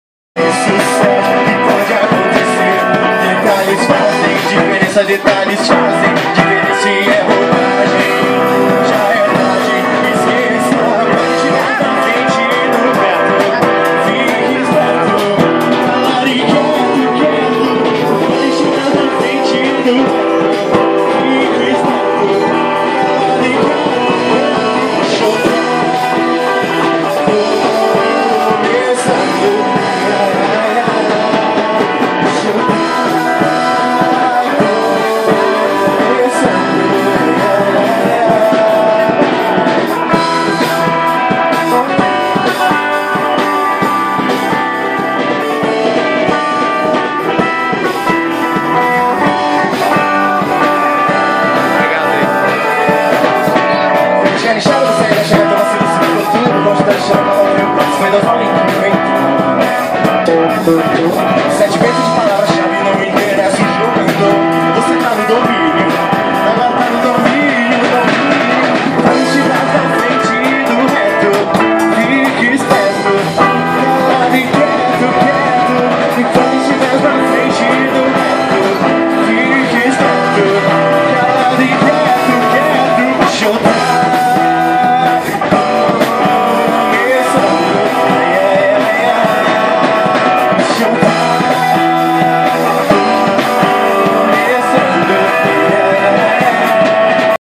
Banda Offsite dando uma canja para a família Baden
Os garotos da banda Offsite alegraram a tarde junto com papai noel e a criançada na cervejaria Baden Baden.